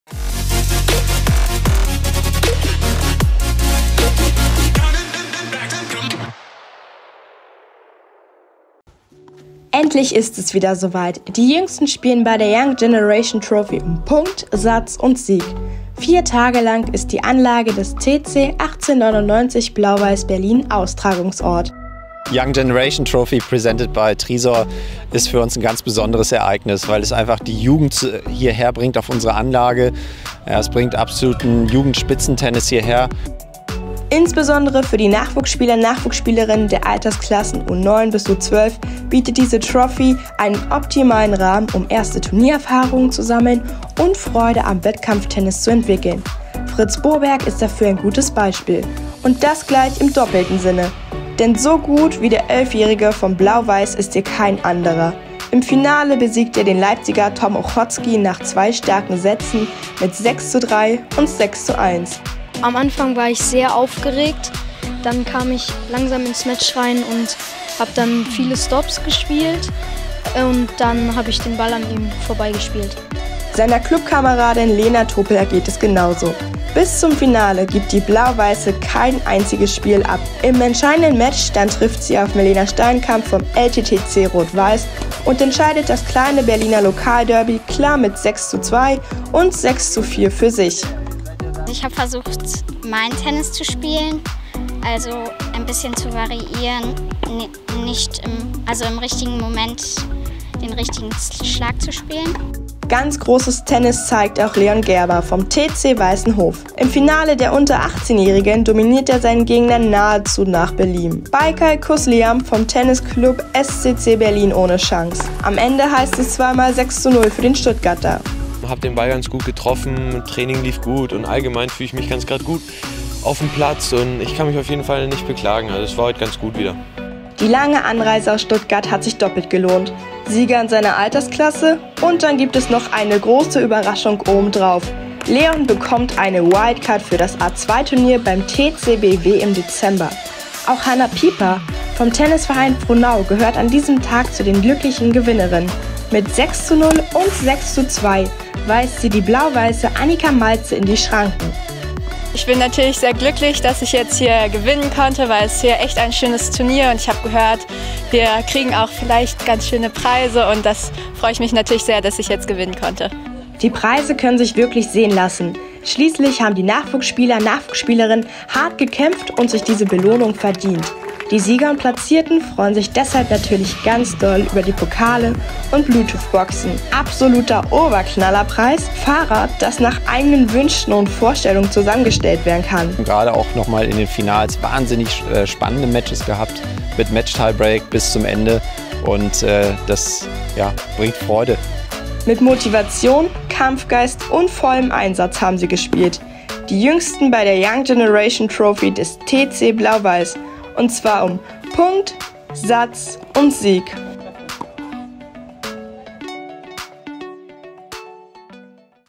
HSTV Throwback des Monats: Young Generation Trophy
Endlich ist es wieder soweit. Die Jüngsten spielen bei der Young Generation Trophy um Punkt, Satz und Sieg. Vier Tage lang ist die Anlage des TC 1899 Blau-Weiss Berlin ein Austragungsort voller Spiel, Spaß und Spannung.